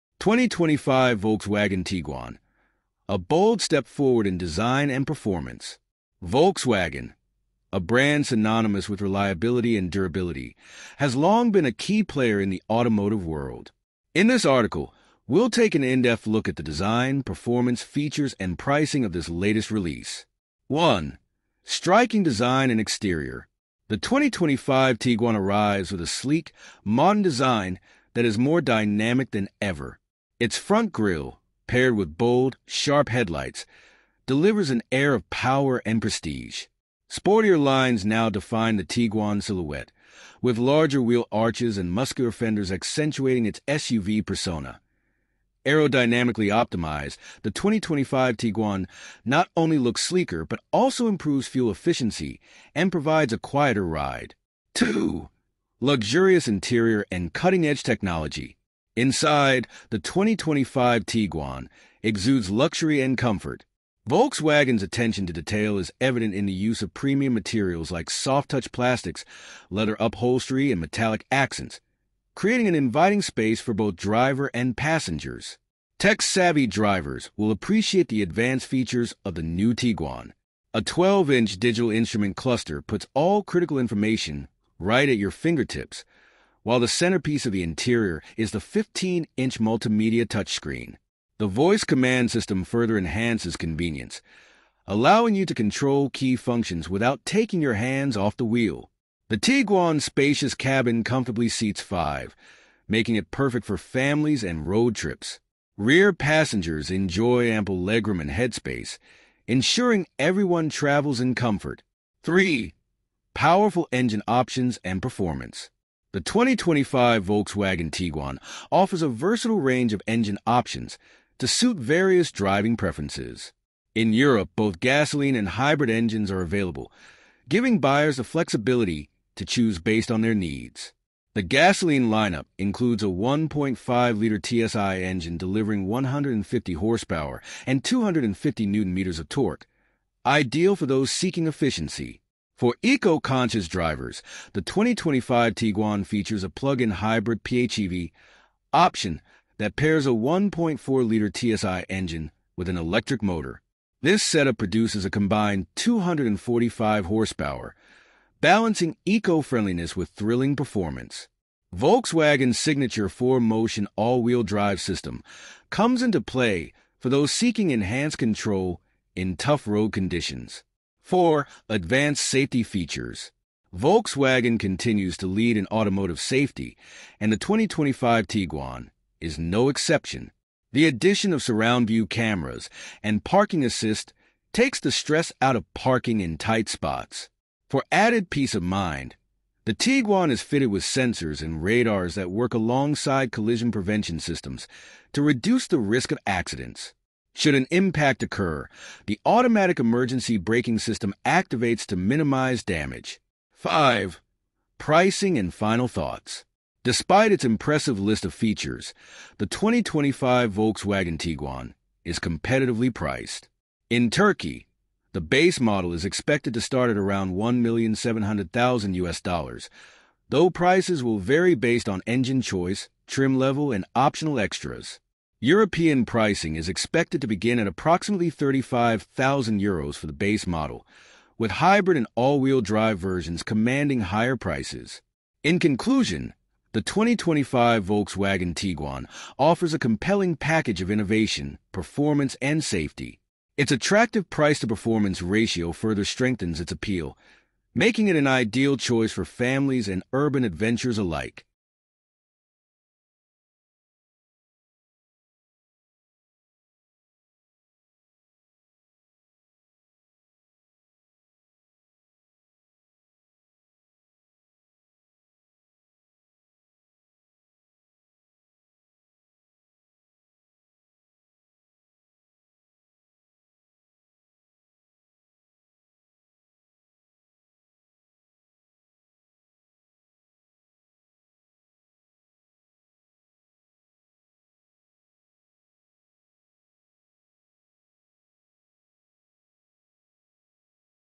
2025 Volkswagen Tiguan Takes on Honda CRV in EPIC Showdown!
Get ready for the all-new 2025 Volkswagen Tiguan! In this video, we’ll explore the sleek design, luxurious interior, cutting-edge technology, powerful engine options, and advanced safety features of this remarkable SUV. Whether you're looking for performance, comfort, or eco-friendly options, the 2025 Tiguan has something for everyone. Plus, we’ll cover pricing details, helping you decide if this bold and modern SUV is right for you!

0:30 - Striking Exterior Design
1:45 - Luxurious Interior & Tech
3:00 - Engine Options & Performance
4:15 - Advanced Safety Features
5:20 - Pricing & Final Thoughts

Highlights: 🚘 Striking, aerodynamic design
🛋️ Luxurious interior with high-end materials
📱 15-inch multimedia touchscreen & voice command
⚡ Gasoline & Plug-in Hybrid (PHEV) engine options
🛡️ Advanced safety features: surround-view cameras, parking assist
💰 Competitive pricing: starting at 1,700,000 USD in Turkey, €35,000 in Europe

Watch as the 2025 Volkswagen Tiguan goes head-to-head with the Honda CRV in an epic showdown! Check out the exterior, specs, and review of the Volkswagen Tiguan allspace 2025 in this thrilling comparison video.